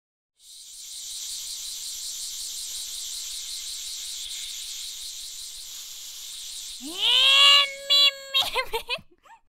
セミの鳴き声（全部俺ver）
最初から最後まで私です。15分練習した

使わせて頂いたフリー素材
・いらすとや 様

普段はゲーム配信や歌ってみたの投稿をしてます‼️